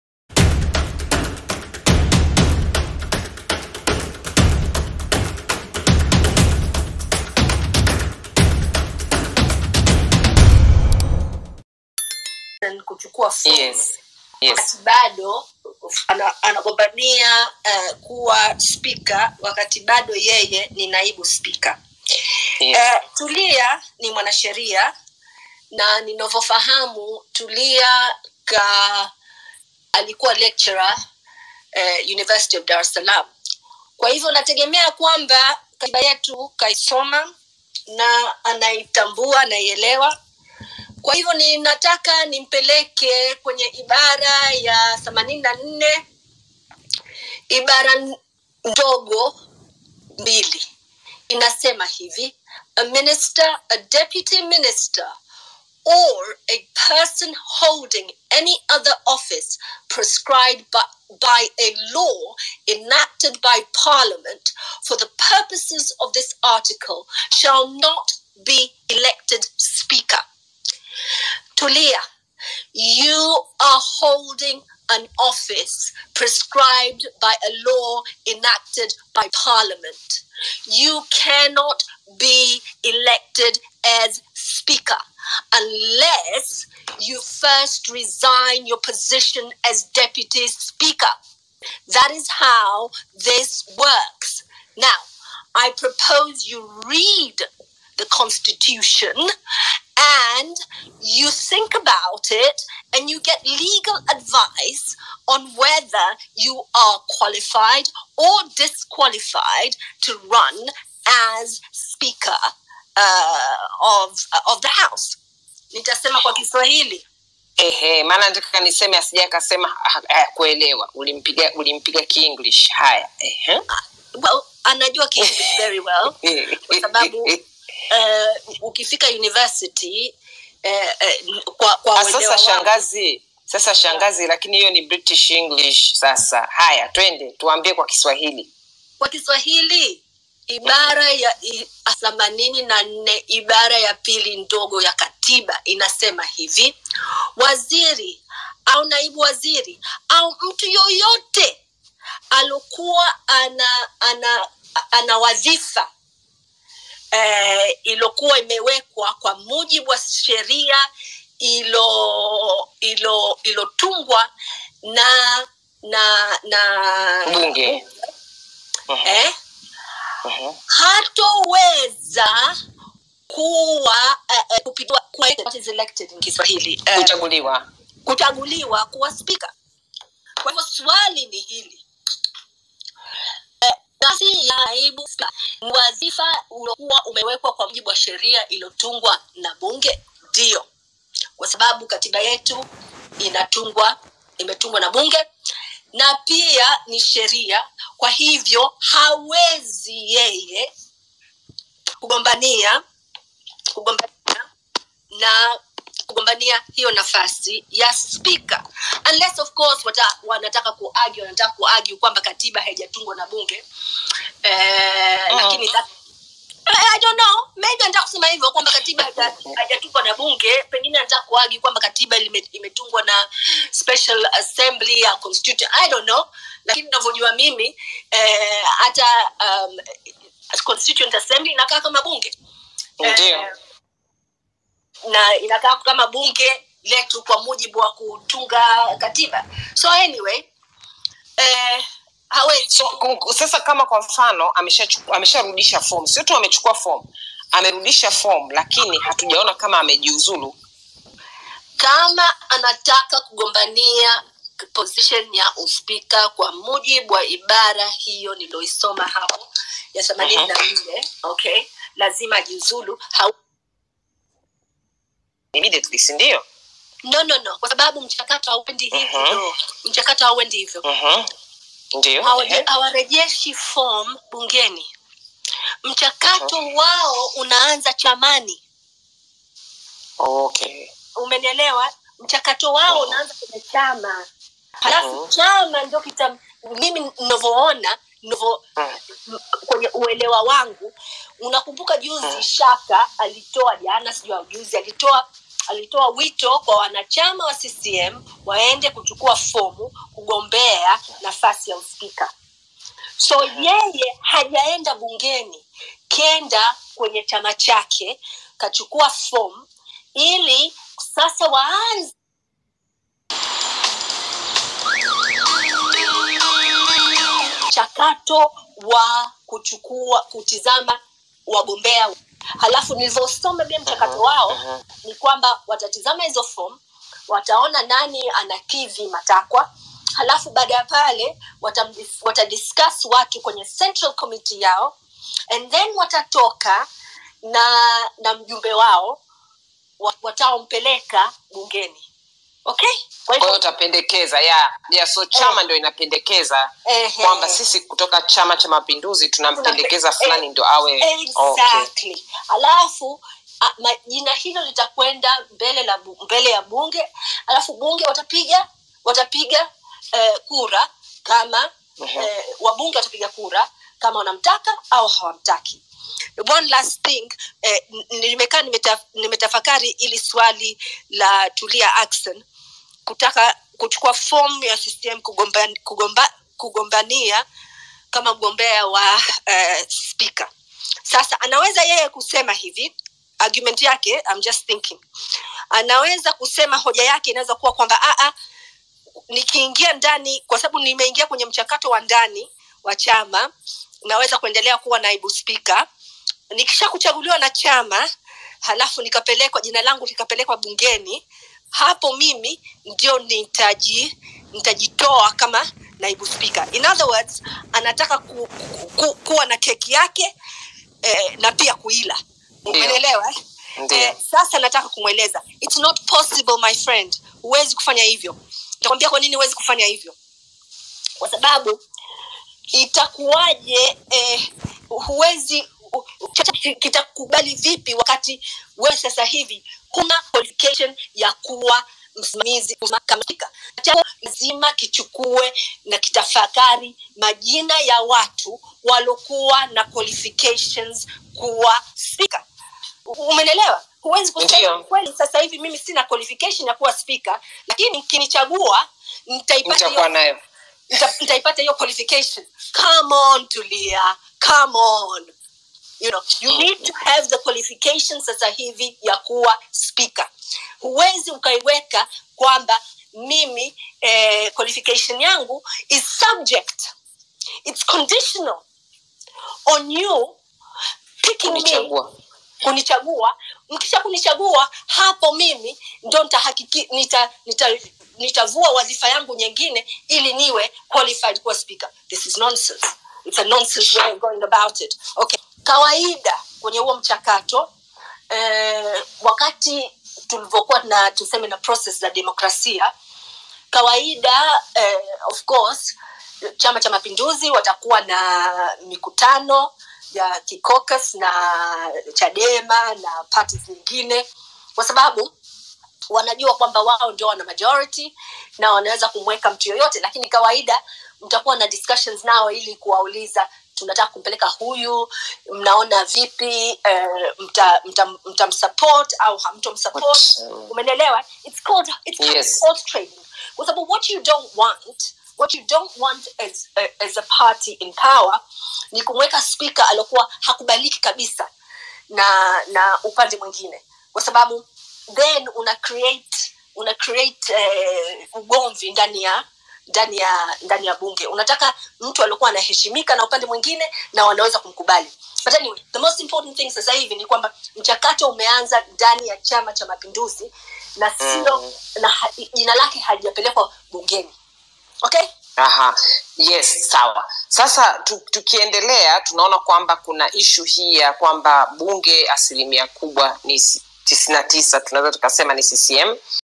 🔴BREAKING
🔴BREAKING NEWS:TAMKO RASMI NAIBU SPIKA AME JIUZURU MUDA HUU ...